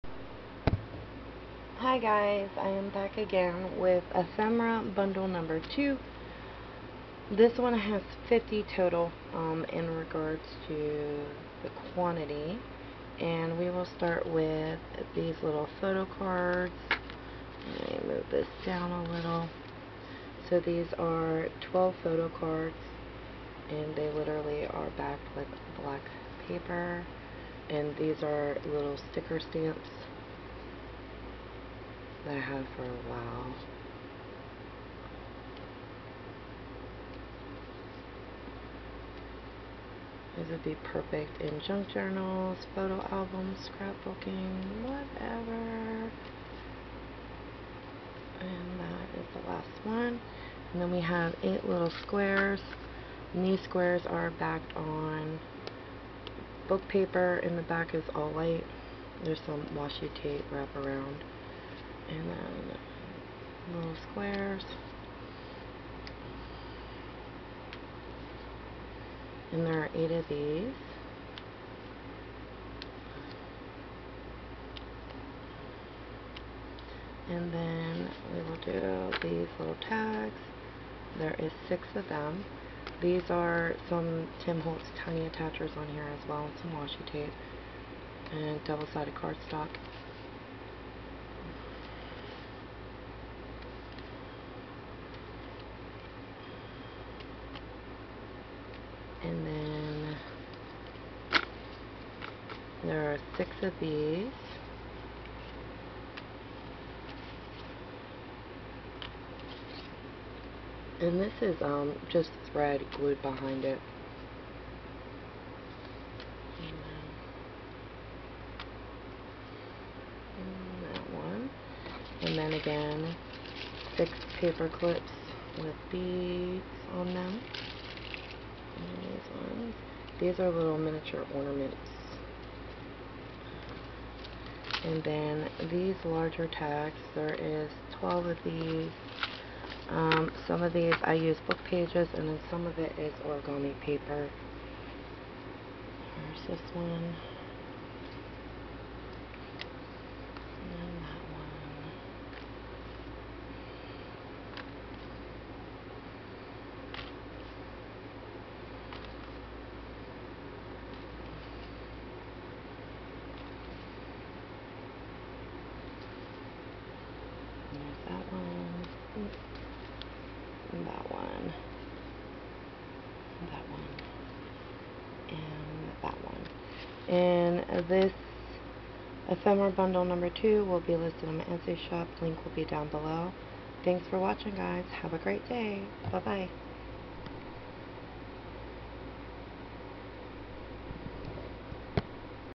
Ephemera bundle #2
This ephemera bundle kit comes with
10 photo cards
8 collage squares
6 larger tags
6 smaller index cards
12 large index cards
6 paper clips with beads
listed in my etys shop